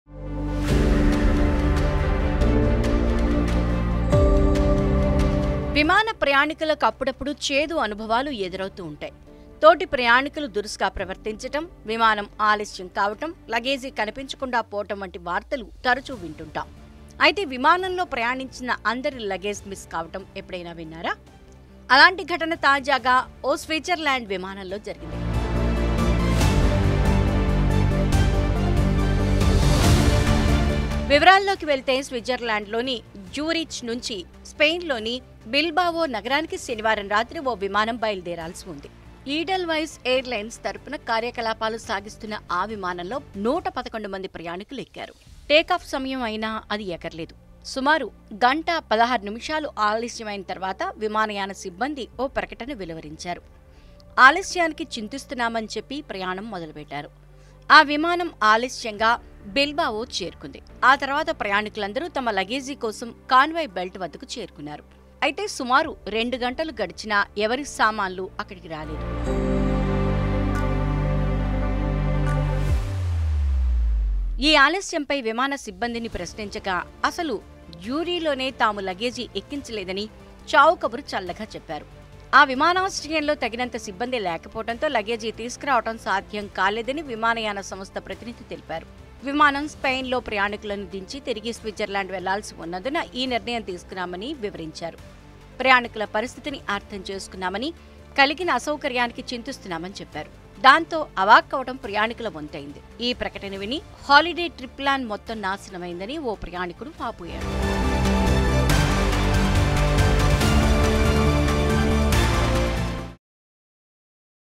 షాక్‌ తిన్న ప్రయాణికులు - TV9
విమాన ప్రయాణికులకు అప్పుడప్పుడూ చేదు అనుభవాలు ఎదురవుతుంటాయి. తోటి ప్రయాణికులు దురుసుగా ప్రవర్తించడం, విమానం ఆలస్యం కావడం, లగేజీ కనిపించకుండా పోవడం వంటి వార్తలు తరచూ వింటుంటాం. అయితే విమానంలో ప్రయాణించిన అందరి లగేజీ మిస్‌ కావడం ఎప్పుడైనా విన్నారా? అలాంటి ఘటన తాజాగా ఓ స్విట్జర్లాండ్‌ విమానంలో జరిగింది.

Credit:Sarada/Producer